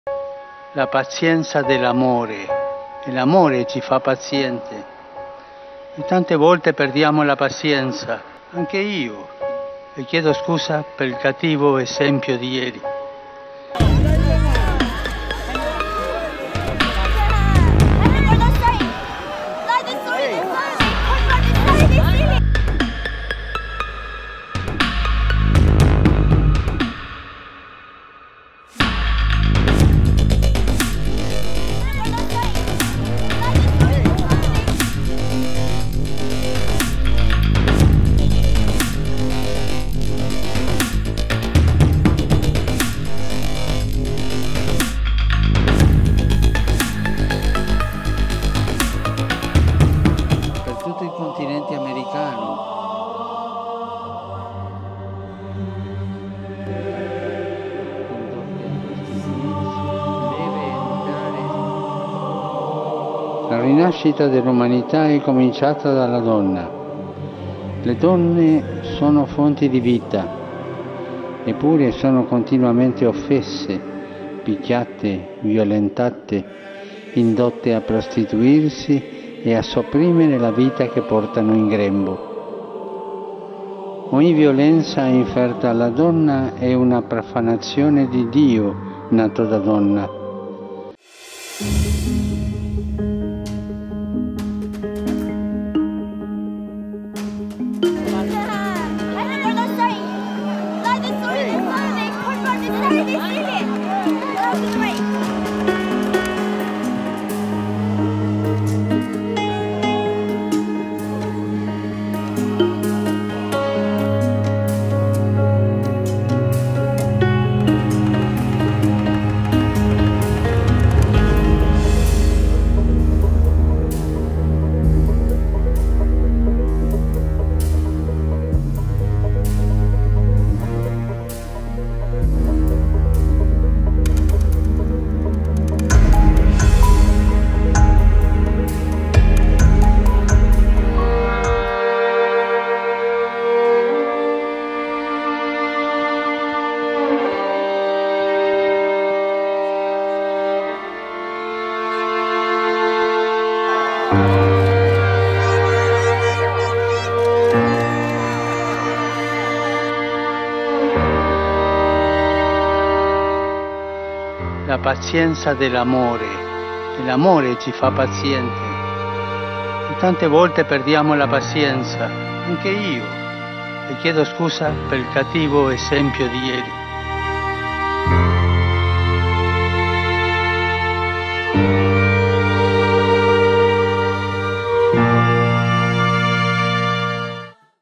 El papa Francisco se disculpa: “Muchas veces perdemos la paciencia”
¡Mea culpa! “Muchas veces perdemos la paciencia, también yo. Pido perdón por el mal ejemplo”, señaló el papa Francisco desde la ventana del palacio apostólico. El Sumo Pontífice, también condenó con firmeza la violencia y las ofensas que sufren las mujeres.